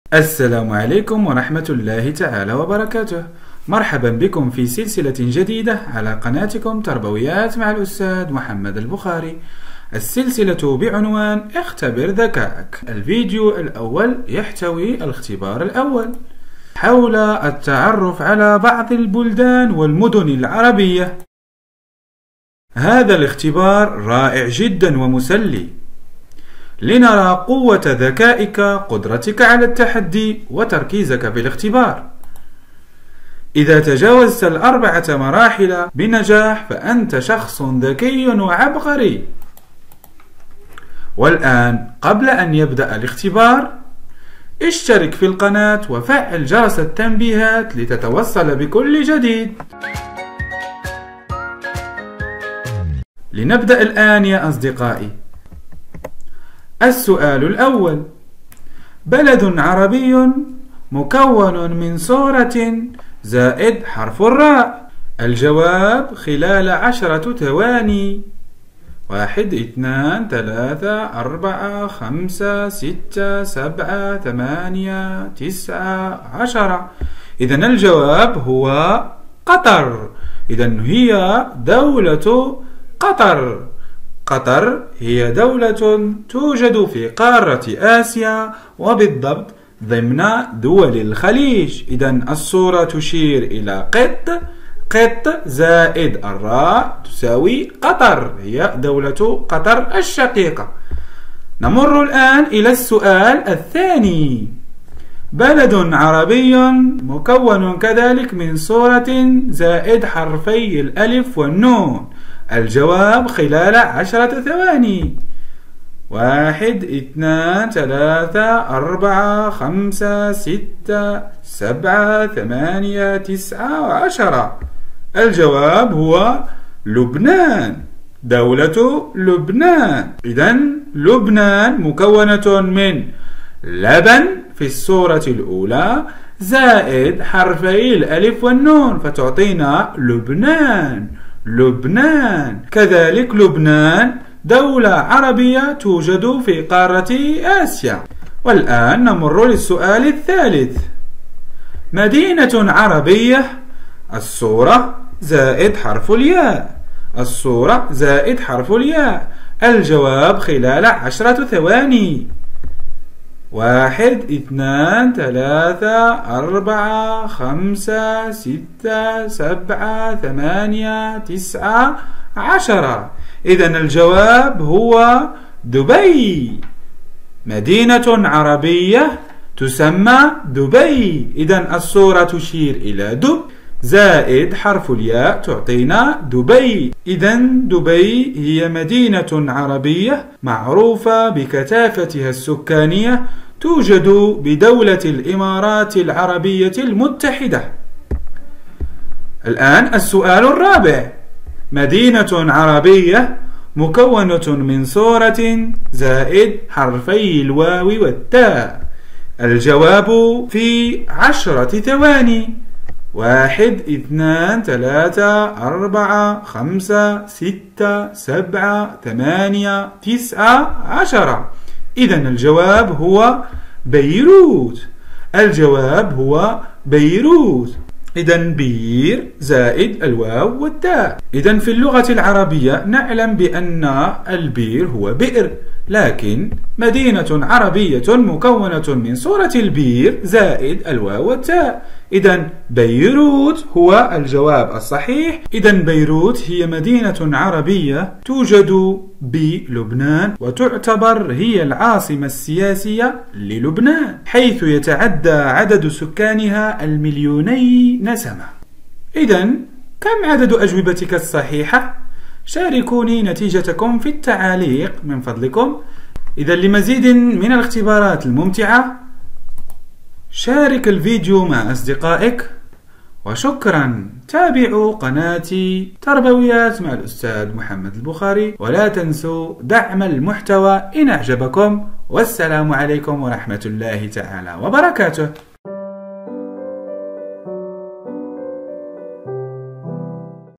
إذا تجاوزت المرحلة 4 فأنت حقا عبقري يا صديقي !! - الاختبار الأول من سلسلة "اختبر ذكائك" -
إذا تجاوزت المرحلة 4 فأنت حقا عبقري يا صديقي !!
مرحبا بكم في سلسة جديدة على قناتكم "تربويات مع الأستاذ محمد البخاري"
السلسلة بعنوان " اختبر ذكائك "